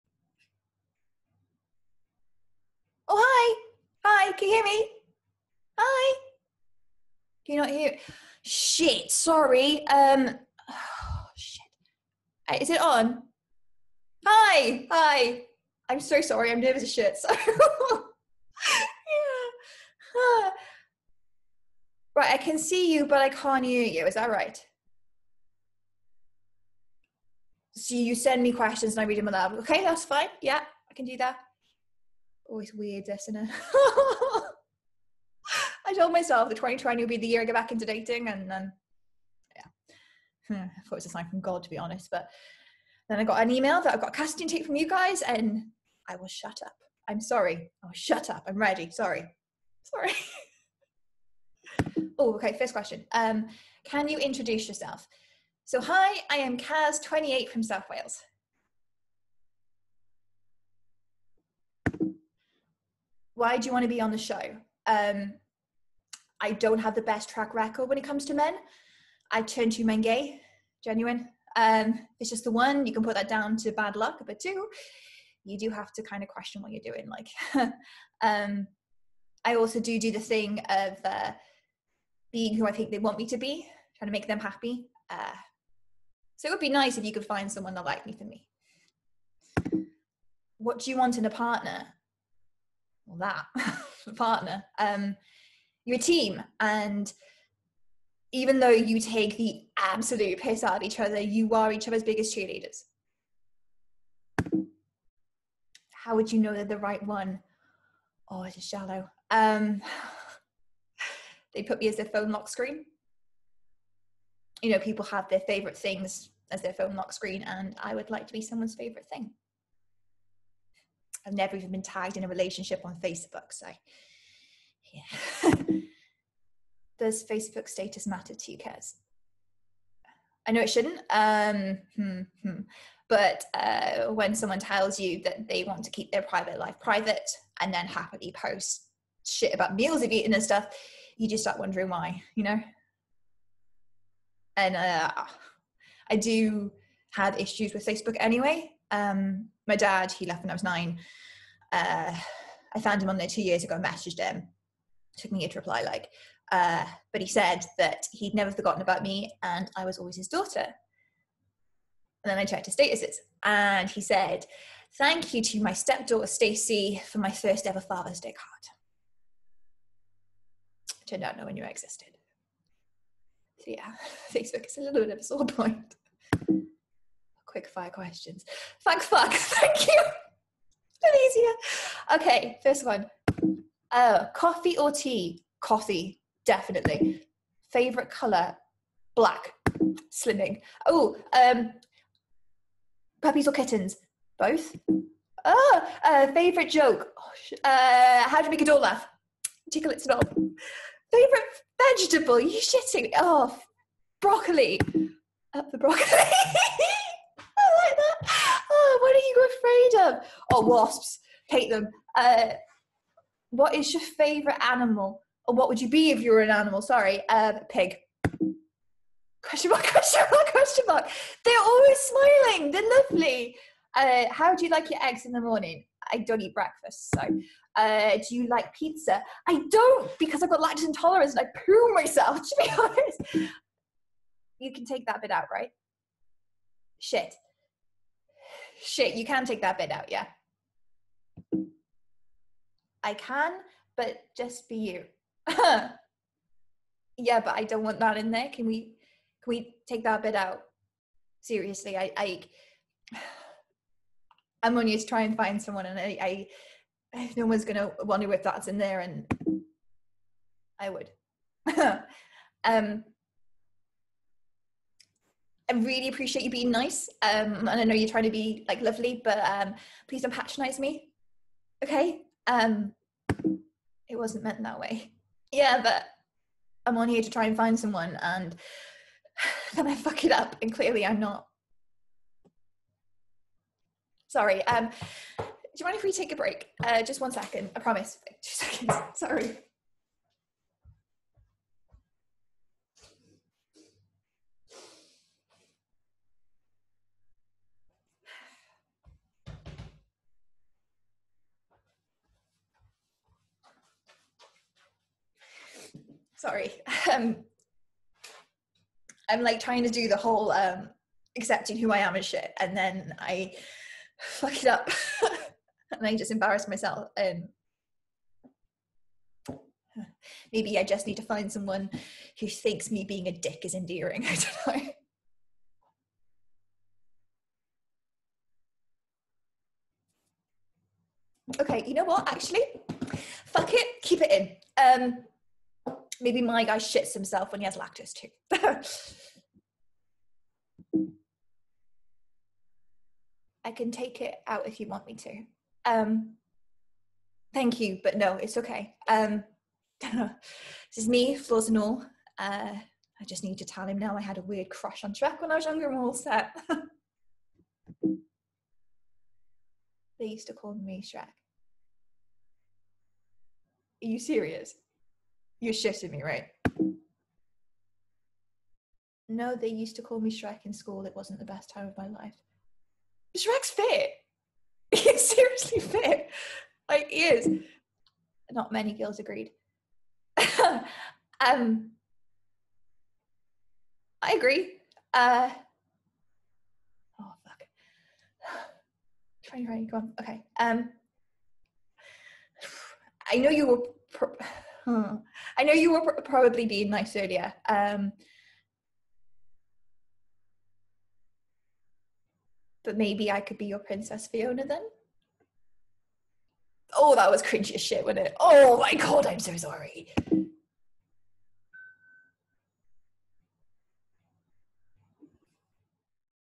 Love Is Blind monologue
Kaz films a remote casting tape for a reality dating show.
Originally commissioned as one of Sherman Theatre's TEN series.